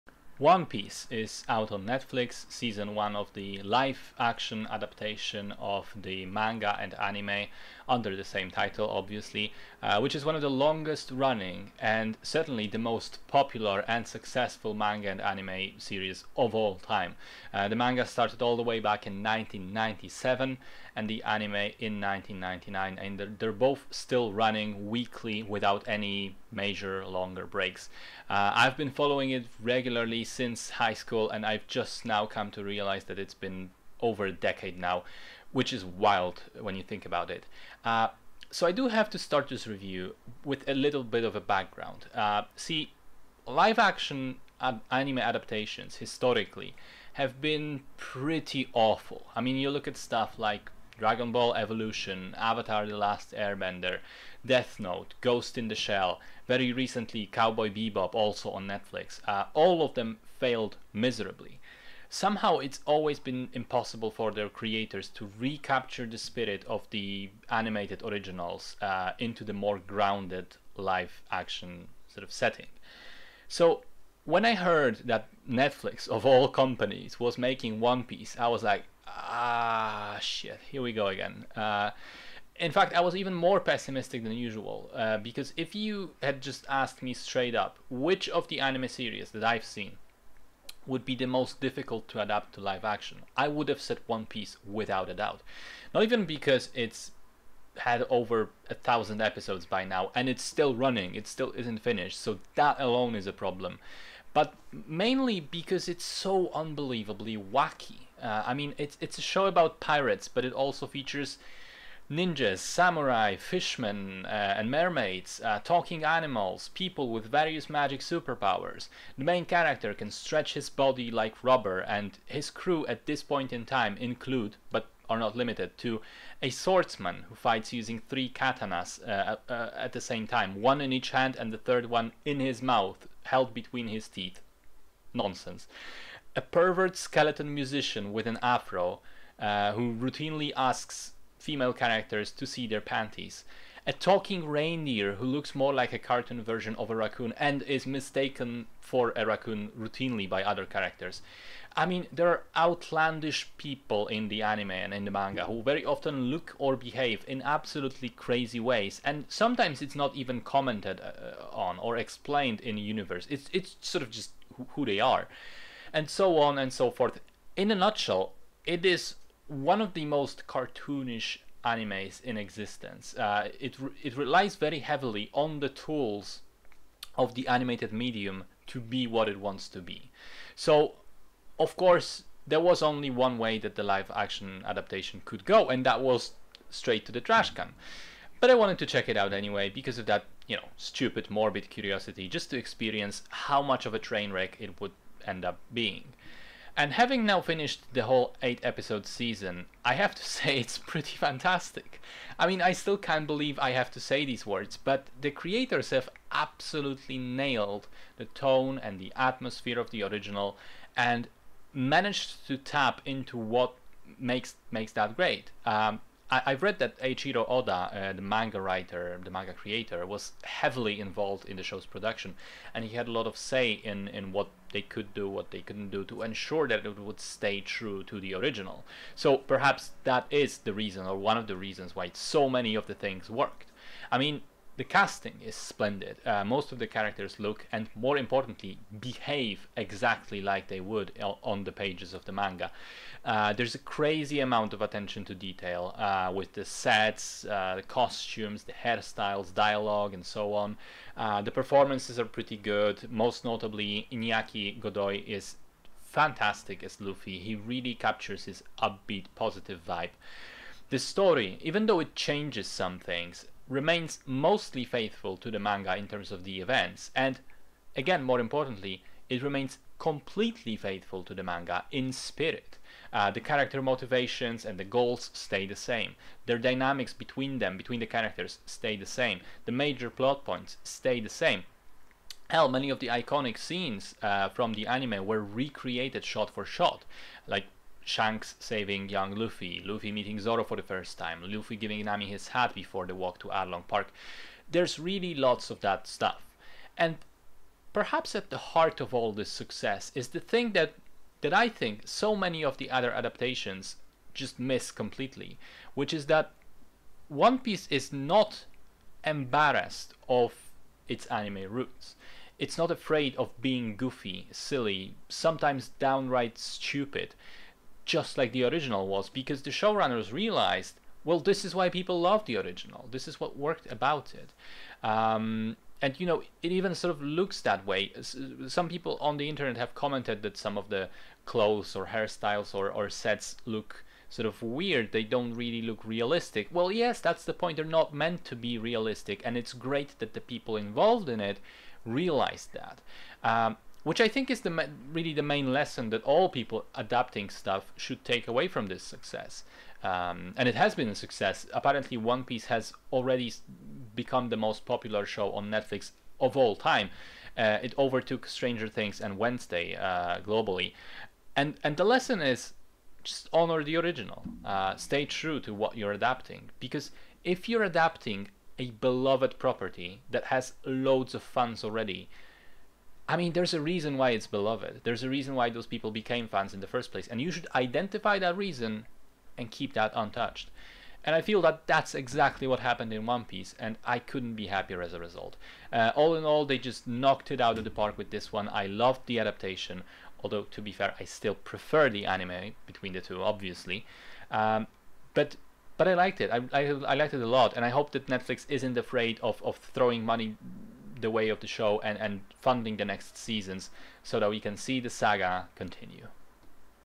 One Piece (season 1) - review
In a seafaring world, a young pirate captain sets out with his crew to attain the title of Pirate King, and to discover the mythical treasure known as 'One Piece.'